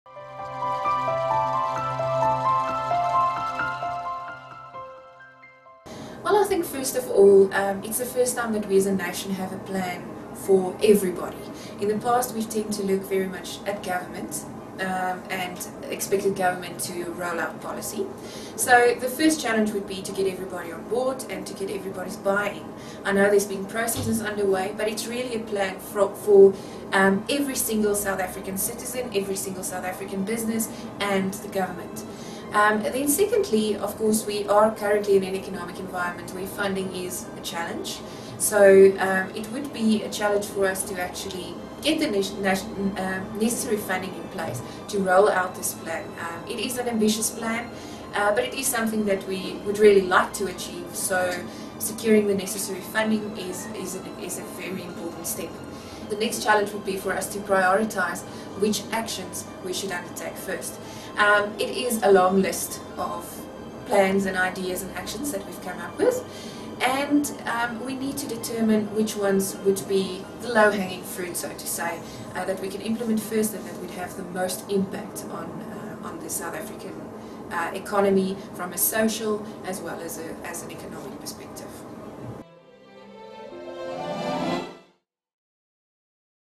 Economic perspective on the NDP - Lullu Krugel, KPMG Senior Economist
Securing funding is one of the biggest challenges facing the implementation of the National Development plan.